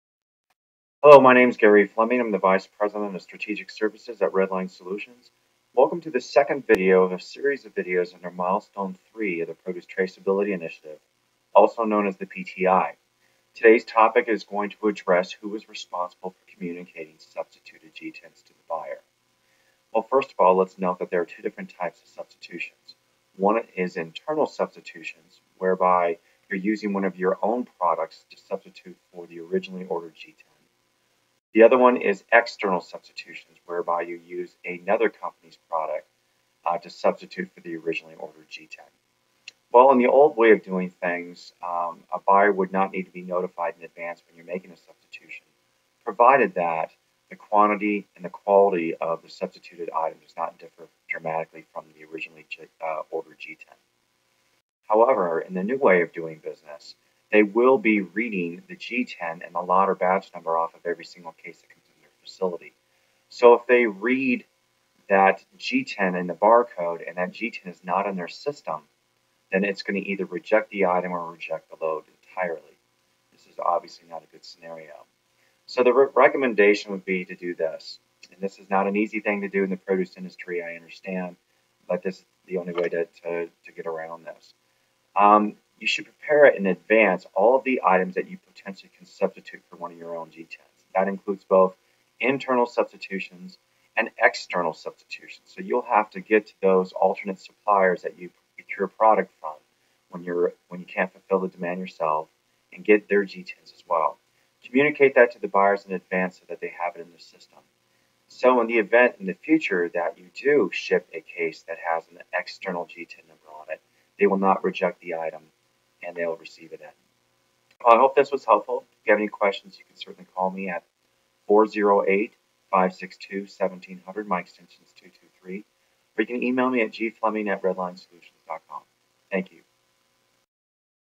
Produce Traceability Milestone 3 Part 2: Communicating Substituted GTINs
PTI Milestone 3

This video addresses how to communicate GTINs for those products you substituted for the originally ordered GTIN and who is responsible for doing so.